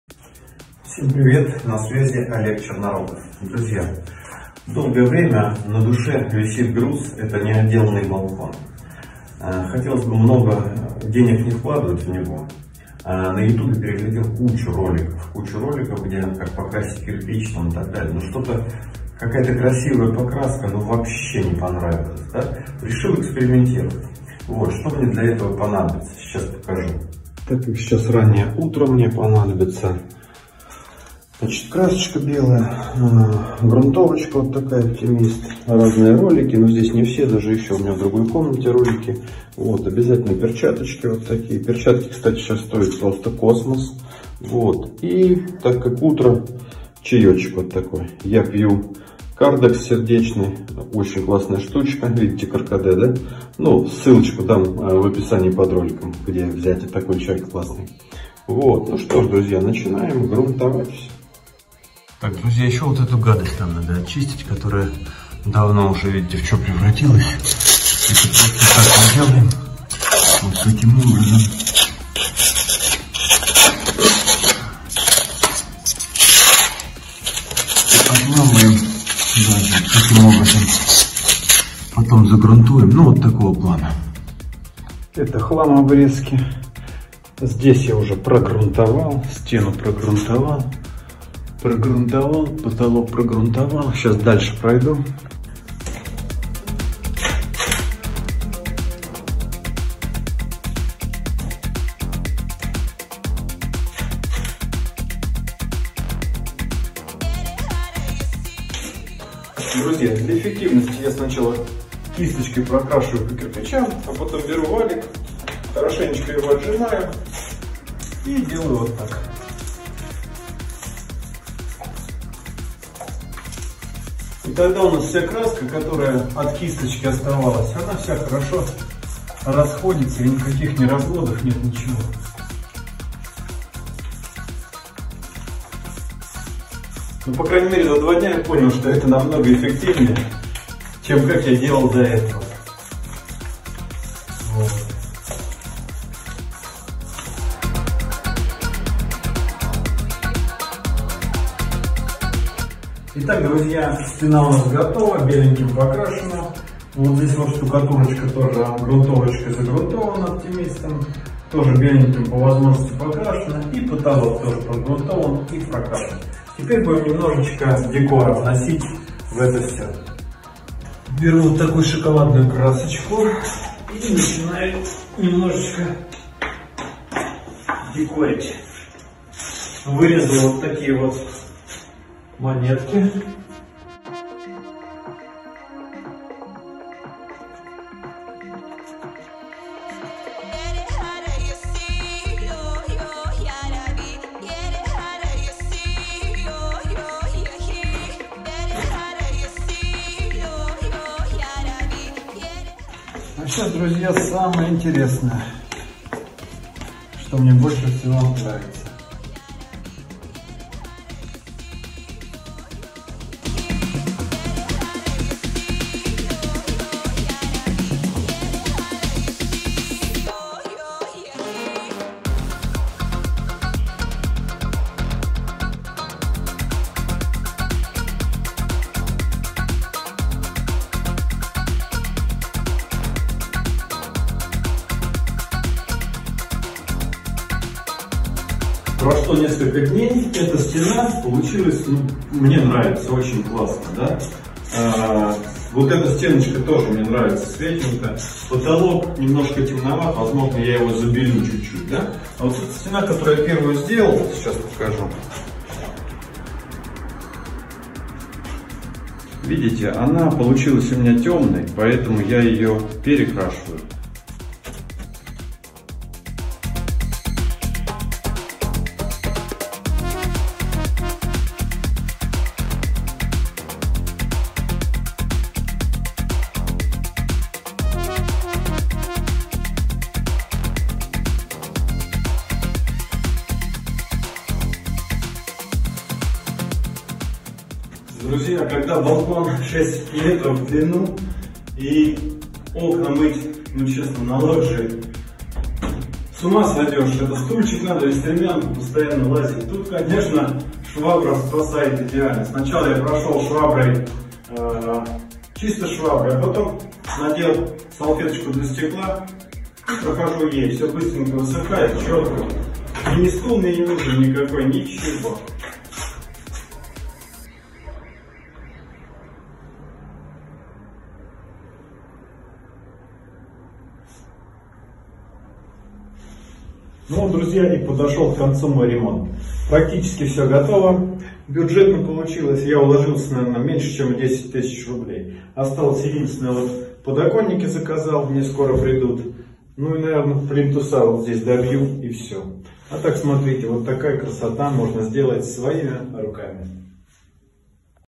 Бюджетный ремонт балкона своими руками I Отделка лоджии I идея для интерьера балкона
больше 5 лет мне не хватало времени и сил дотянуться до ремонта балкона.

В этом году эта цель была поставлена, и я начал её реализацию. Условия задачи очень просты. Что бы кайфово было летом на балконе и конечно уложиться в небольшую сумму.
Что из этого получилось смотрите сами.

Ах да в видео я пью чай Cardex. Купить его можно в онлайн магазине по ссылке

Если понравилось видео напишите небольшой комментарий.
НЕ понравилось, напишите, что не понравилось, я за конструктивную критику